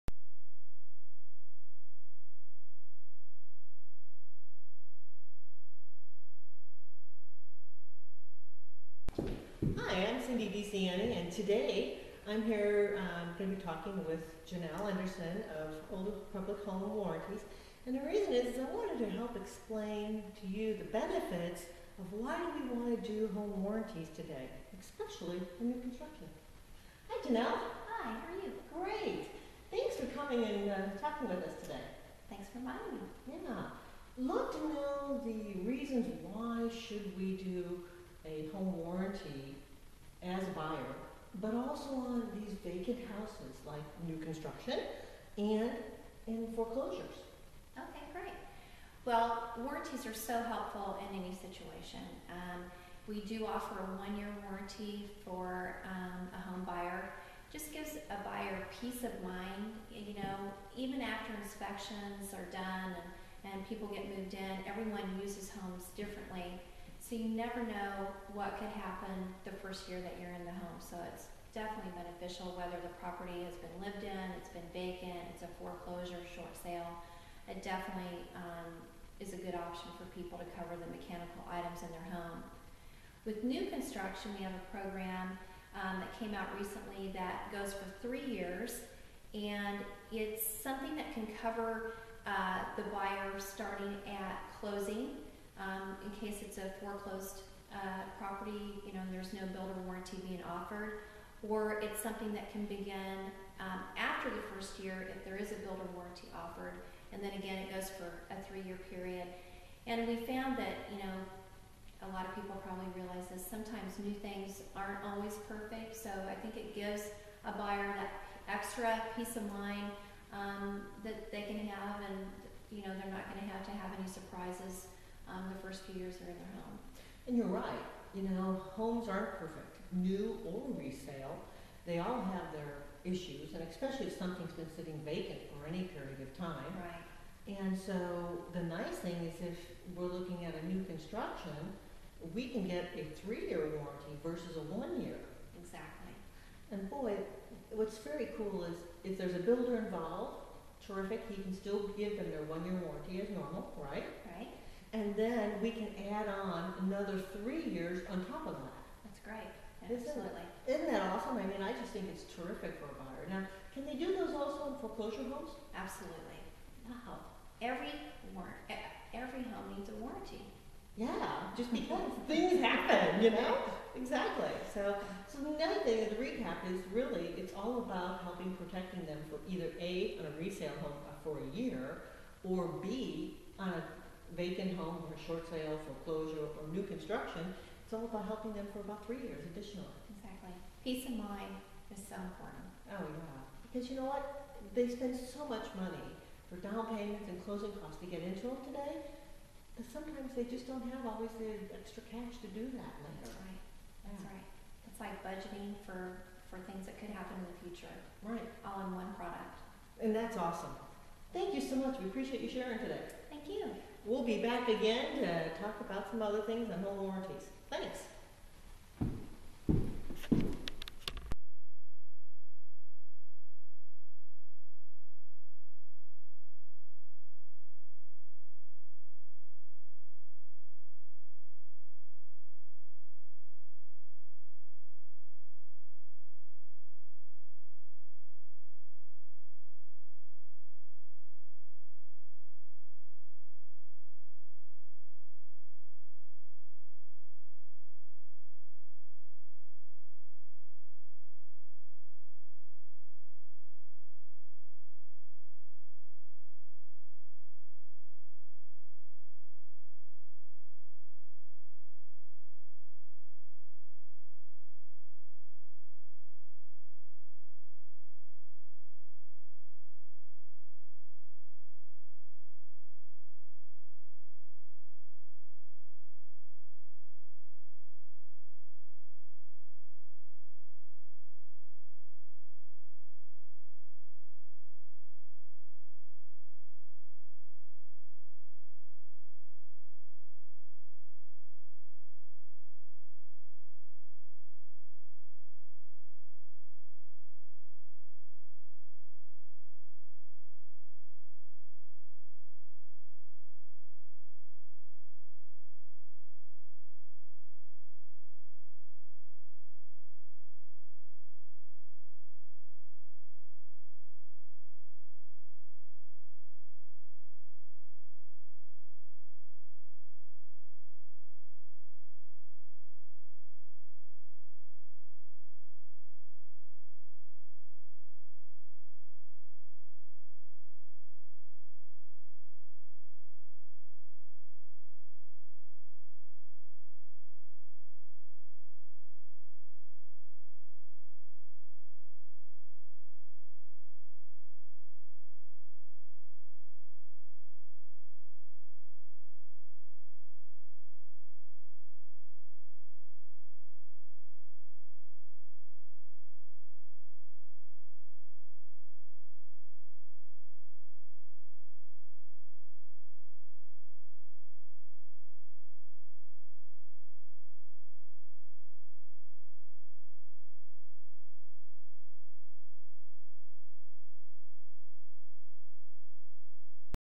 Are you Covered for a Vacant Home? Home Warranty...
Cindy DiCianni of ReMax SL interviews Janell Enderson of Old Republic.  The discussion of the benefits of having a home warranty on vacant homes, new construction, foreclosure homes & short sales homes in Overland Park & Kansas City area,